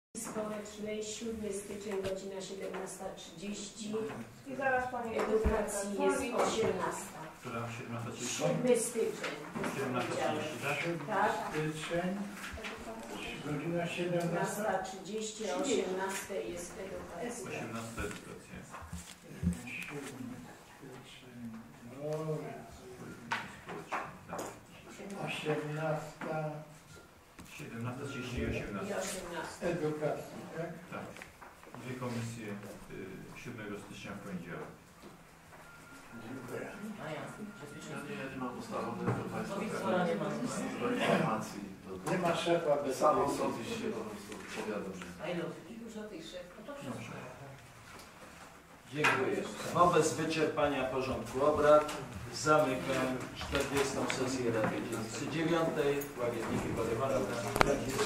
RD_IX_14-12-2012_6_Terminy posiedzeń komisji, zamknięcie sesji
Obrady Rady Dzielnicy IX na XL sesji w dniu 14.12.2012 r. Terminy posiedzeń komisji, zamknięcie sesji.